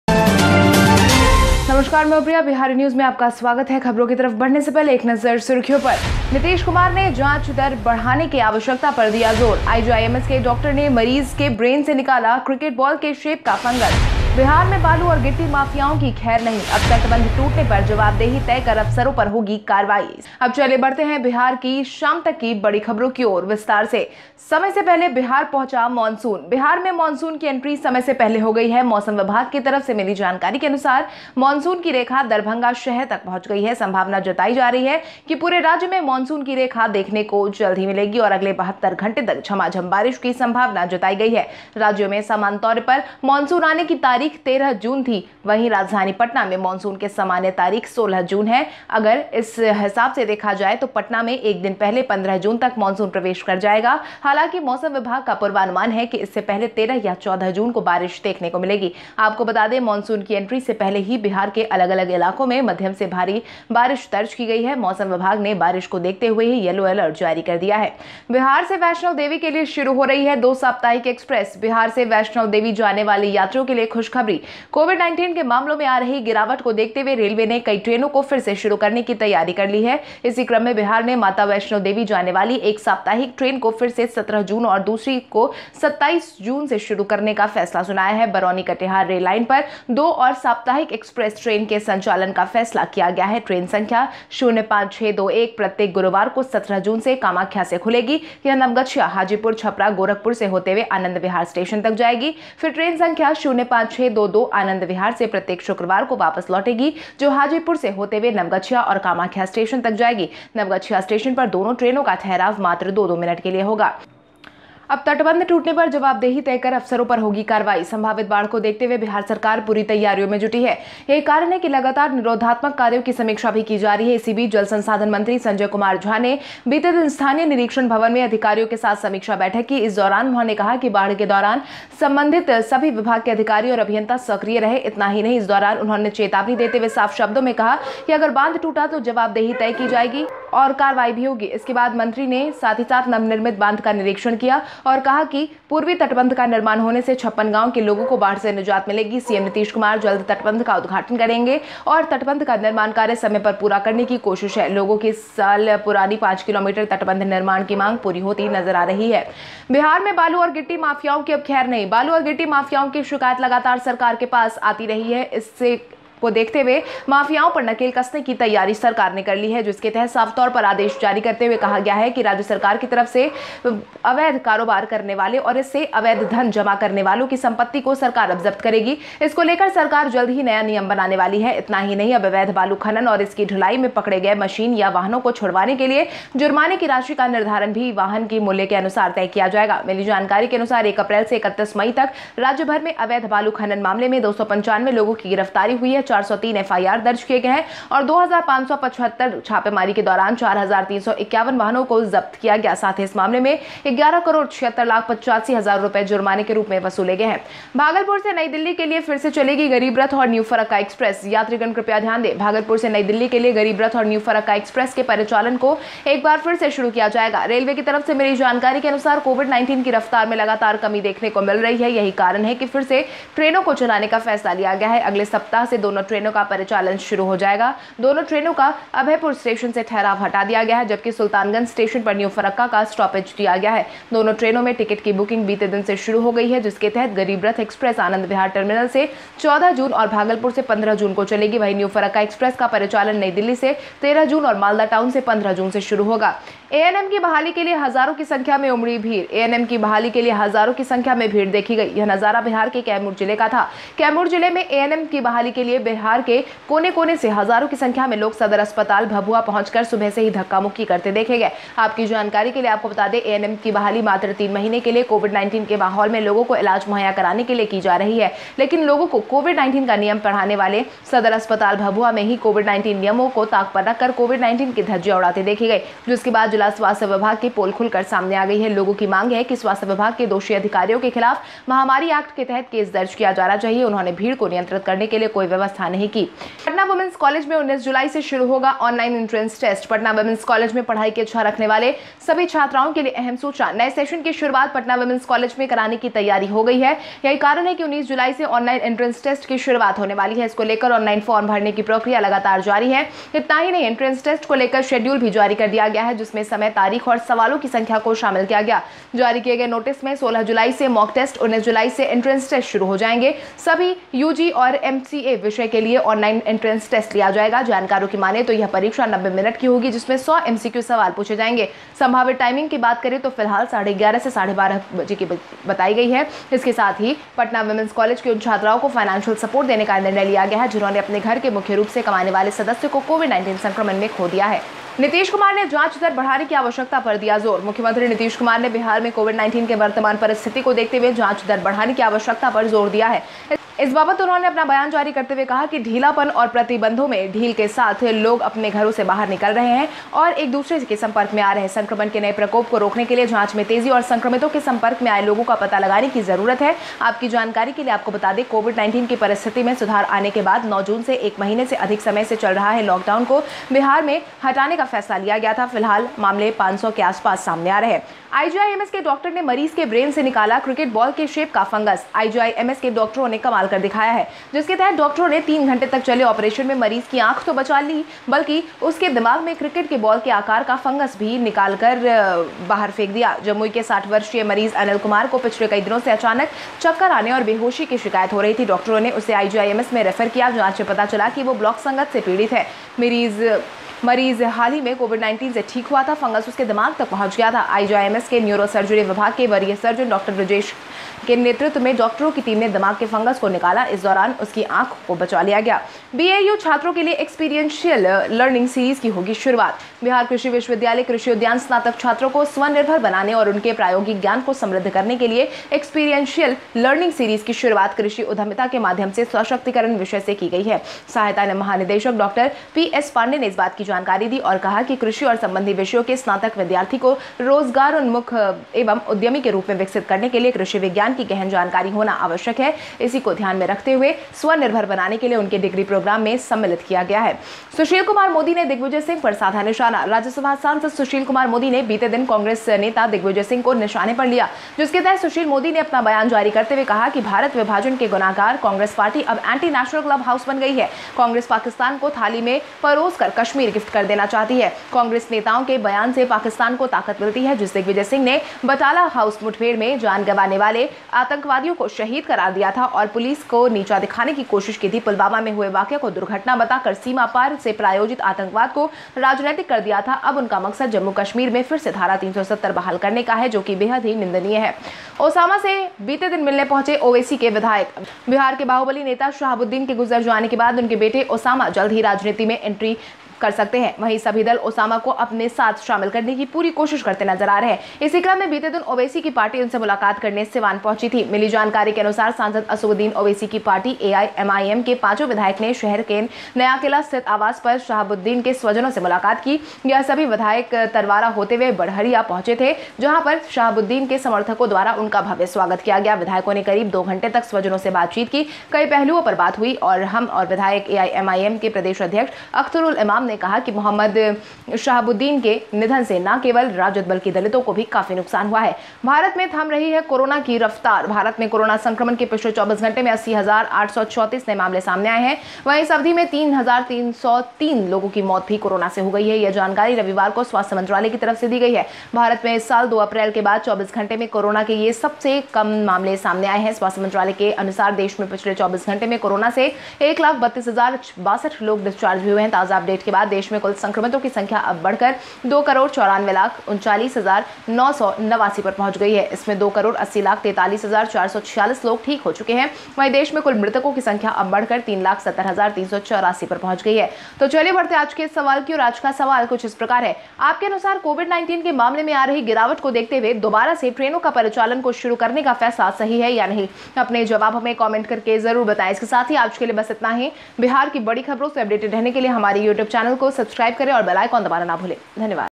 Get Bihar Evening breaking news of 13th June on ANM, Mansoon in bihar, Owaisi, IGIMS.
Latest today Bihar evening news of  13th June on ANM, Mansoon in bihar, Owaisi, IGIMS.

1. Sushil Kumar Modi targeted Digvijay Singh

2. Monsooninbihar reached Bihar before time

3. Thousands gathered for the restoration of ANM

4. Now action will be taken on the officers by fixing the accountability on the breach of the embankment

5. Online entrance test will start in Patna Women's College from July 19

6. Not well for sand and ballast mafia in Bihar
7. GaribRath and New Farakka Express will run again from Bhagalpur to New Delhi

Passengers please note that the operation of Garib Rath and New Farakka Express from Bhagalpur to New Delhi will be started once again, according to the information received from the Railways, there is a continuous decrease in the speed of Kovid-19, this is the reason. That it has been decided to run the trains again. Both the trains will start operating from next week. The stoppage of both the trains has been removed from Abhaypur station while the stoppage of New Farakka has been given at Sultanganj station. Booking has started from the previous day. Under which Garib Rath Express will run from Anand Vihar Terminal on June 14 and from Bhagalpur on June 15, while New Farakka Express will start operating from New Delhi on June 13 and Malda Town from June 15.

8. Bhojpuri actress AksharaSingh reached ASP office

9. Corona's speed is stopping in India

10. Owaisi's MLAs came to meet Osama even on the day

11. Today in Bihari Birthday Special we will talk about Shekhar Suman

12. Nitish Kumar stresses on the need to increase the investigation rate

13. Two Weekly Express is starting from Bihar to Vaishno Devi

14. IGIMS doctor removes cricket ball shaped fungus from patient's brain

15. Experiential Learning Series will be started for BAU students

So let's move on to today's question and today's question is something like this, according to you, whether the decision to start the operation of trains again is right or not, in view of the decline in the case of Kovid-19.